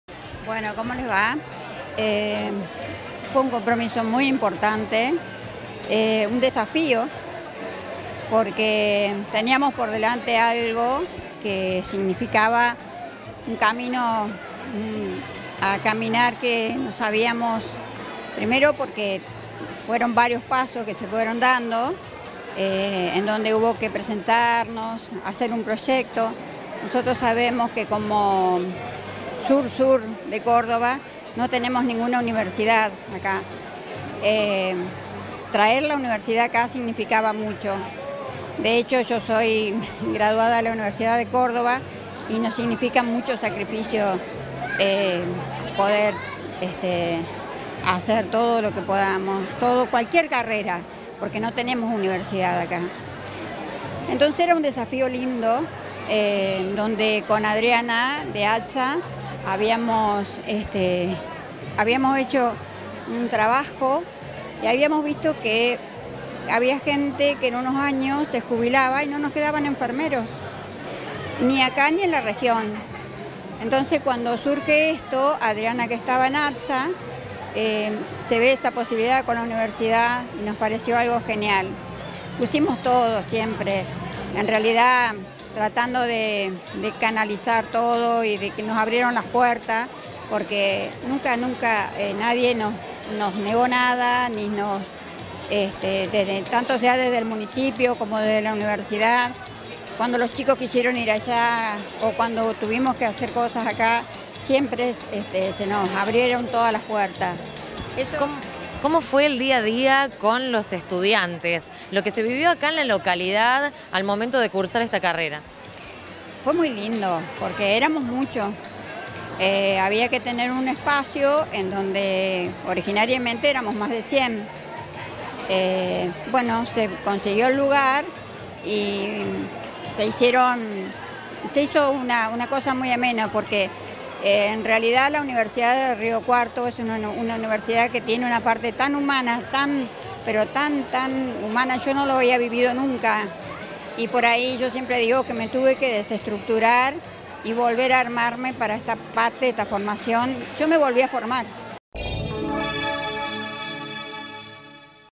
COLACIÓN DE GRADO EN LABOULAYE: ENFERMERÍA TITULÓ A 72 EGRESADOS
PATRICIA SARMIENTO – COORDINADORA LOCAL
Facebook.com/uniriotv
Twitter.com/uniriotv